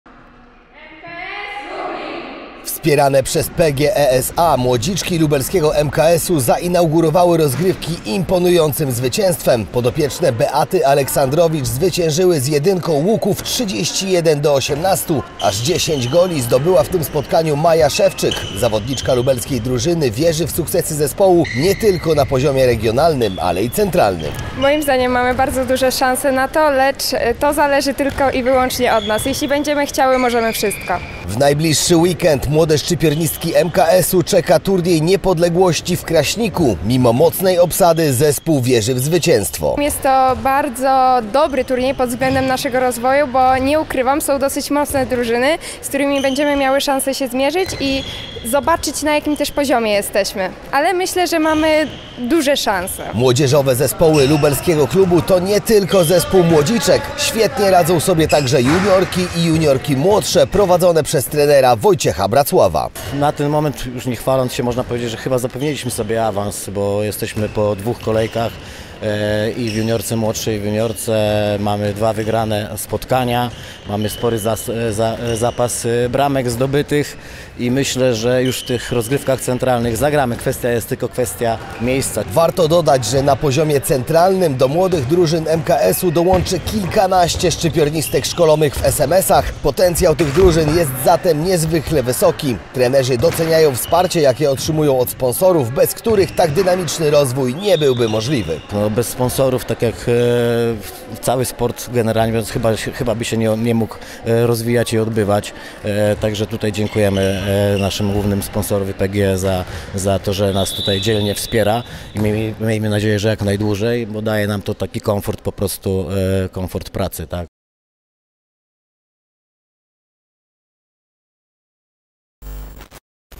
Grupy młodzieżowe MKS FunFloor Lublin | Turniej Niepodległości
Młodzieżowe zespoły biało-zielonych rosną w siłę 💪

Sprawdziliśmy, co słychać u młodziczek, juniorek i juniorek młodszych. Młode szczypiornistki MKS Lublin idą po swoje